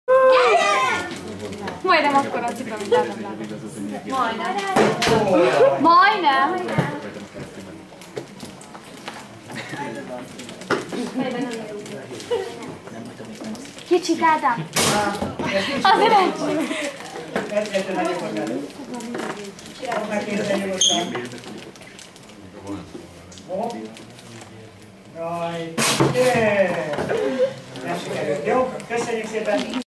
09 Vetélkedő - Cipő az asztalon - Ádám
16 éves a Talent Énekstúdió - Szülinapi Buli március 15-i megemlékezéssel, vetélkedővel, tortázással énekléssel...
Szeged I.H. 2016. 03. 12.